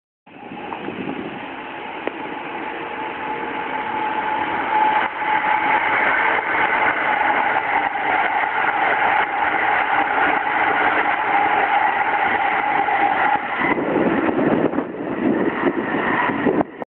鉄道撮影（鎌ヶ谷市制記念公園展望台より）５
京急新1000形アルミ車による普通印旛日本医大行き